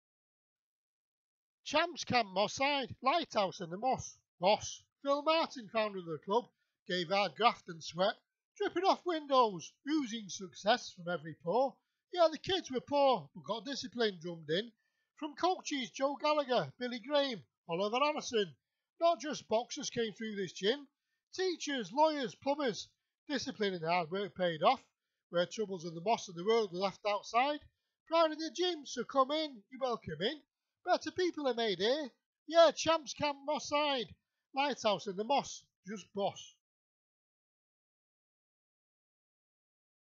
Champs Camp Moss Side Boxing Club
Champs Camp Moss Side Boxing Club, a painting and story by Andrew Alan Matthews andrewalanmatthews.co.uk part of my Moss Side Nostalgia collection